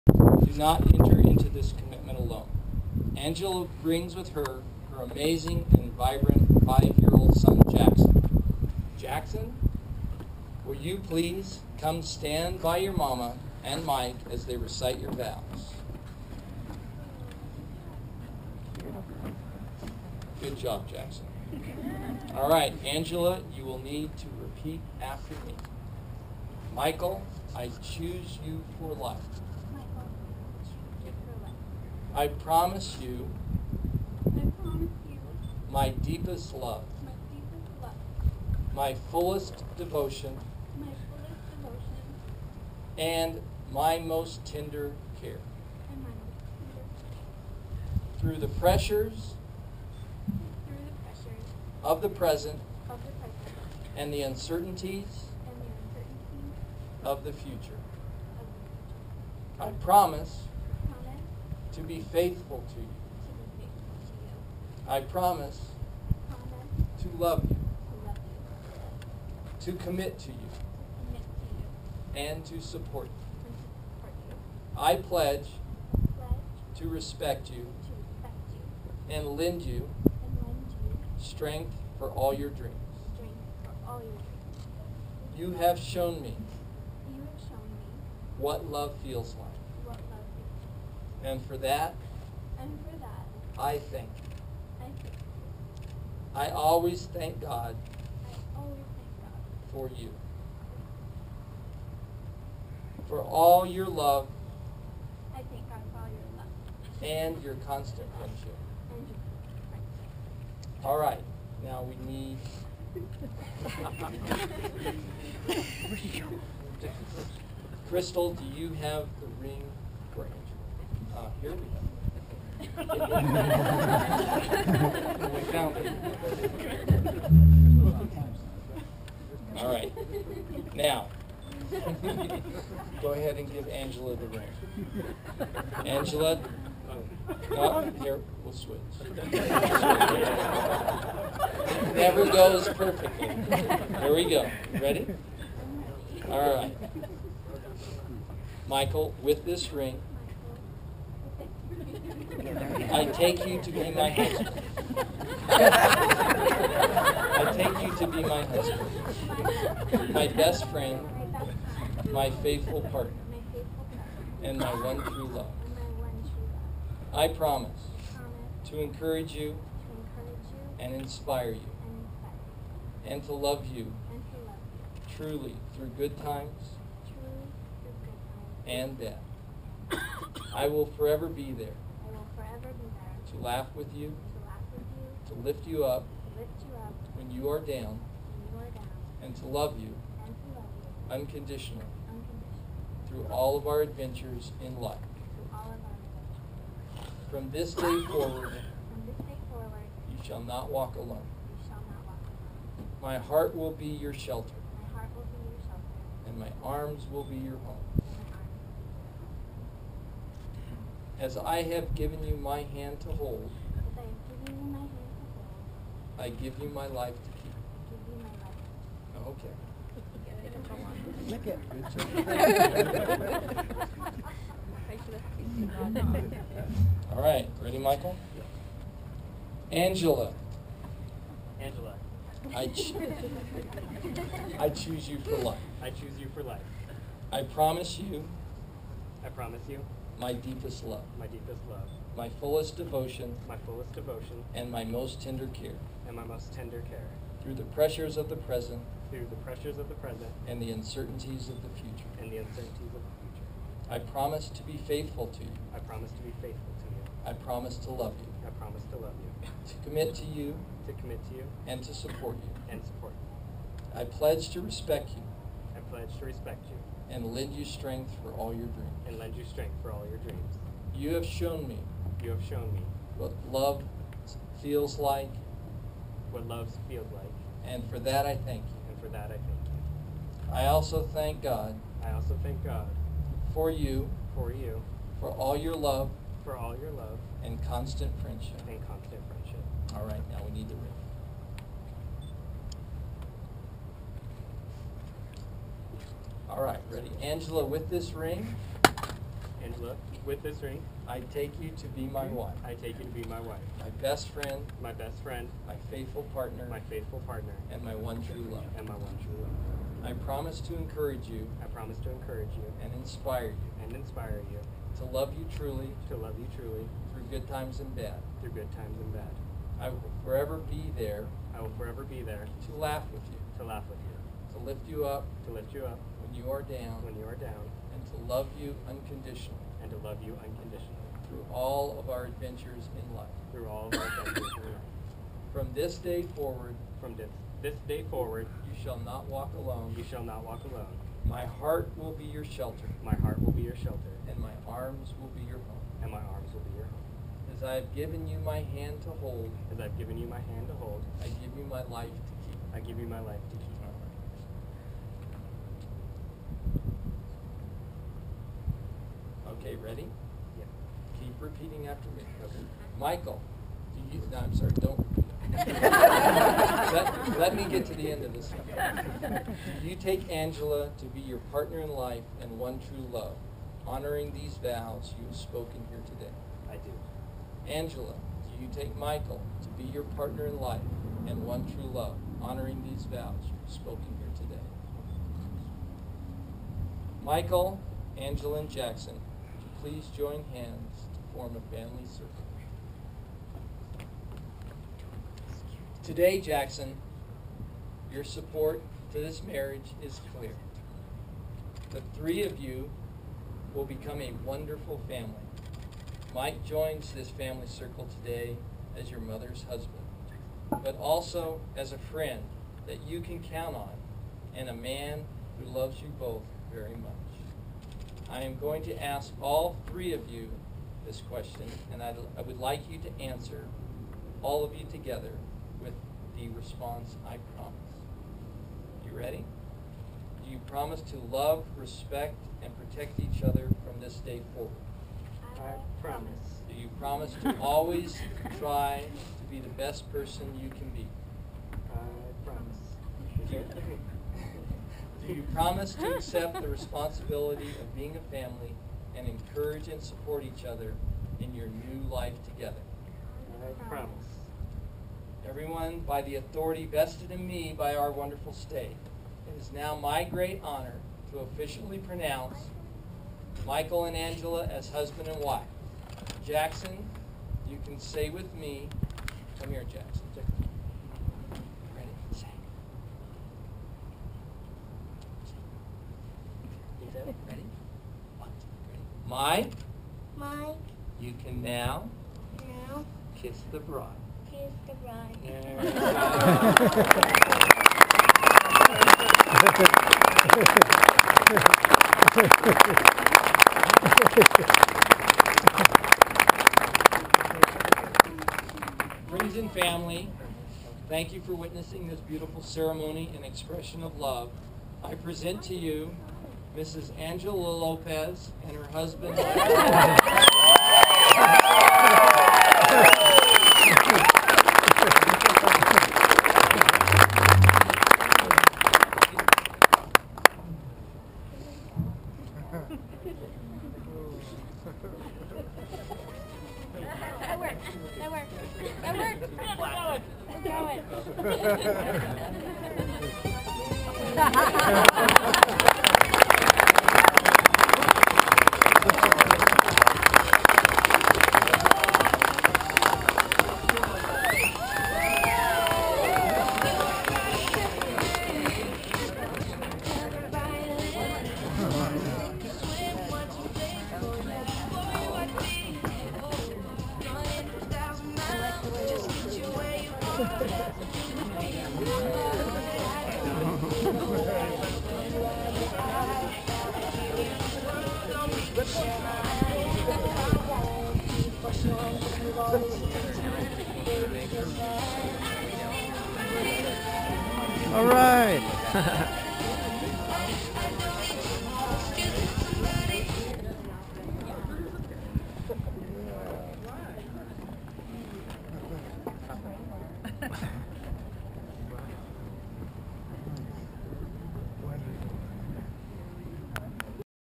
Michael and Angela Lopez Wedding Part 2
This amazing wedding of my niece Angela and Michael took place on top of the Eldorado Hotel in downtown Santa Fe, New Mexico. Due to my stupidity I left my scuba water housing on my gopro camera and took it off in time for this 2nd part you see here.